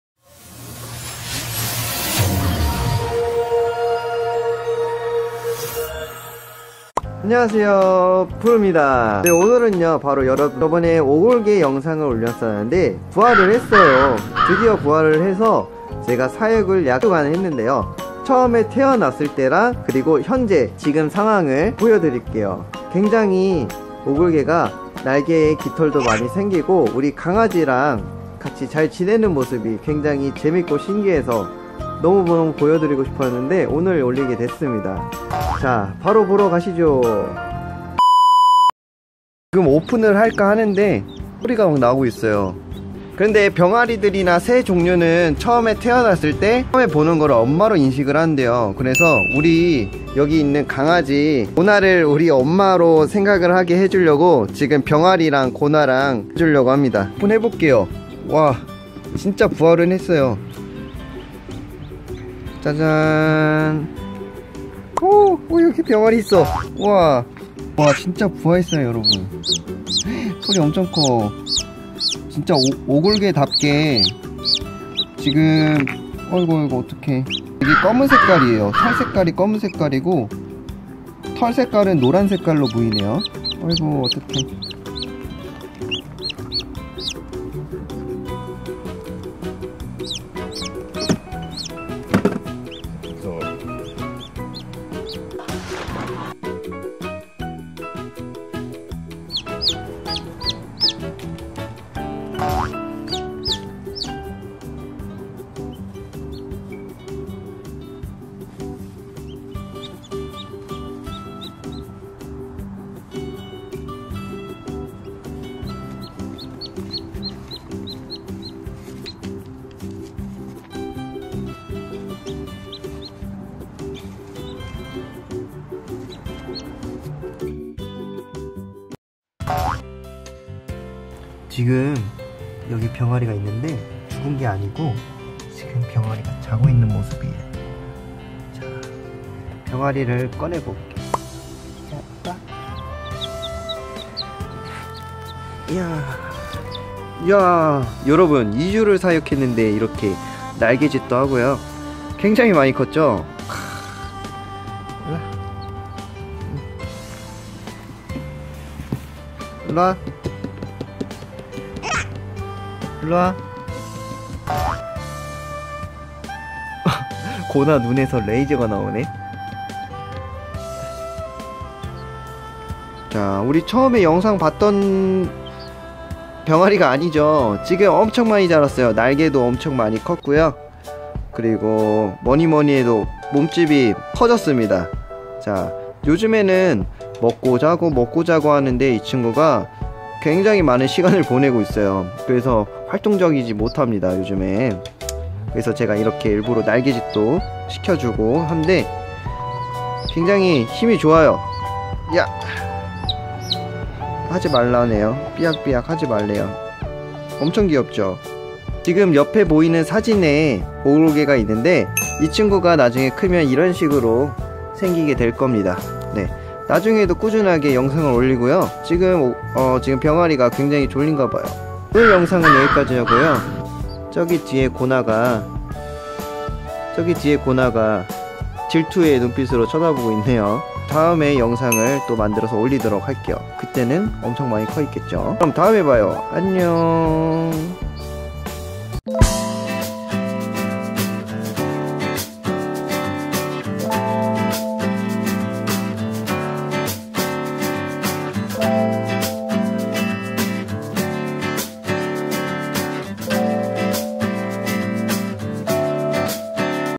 브르)강아지에게 오골계를 보여줬더니...ㄷㄷ
오골계가 드디어 태어났습니다 . 아쉽게도 6마리중에 1마리만 부화했지만 너무나도 기쁘고 정성스럽게 키우려고 합니다. 수명이 7~12년이니깐 제가 고나와 함께 끝까지 최선을 다해서 키울게요!!!

사슴벌레, 장수풍뎅이 ,레오파드게코 , 크레스티드 게코 입양문의
카카오톡 jjung2ekd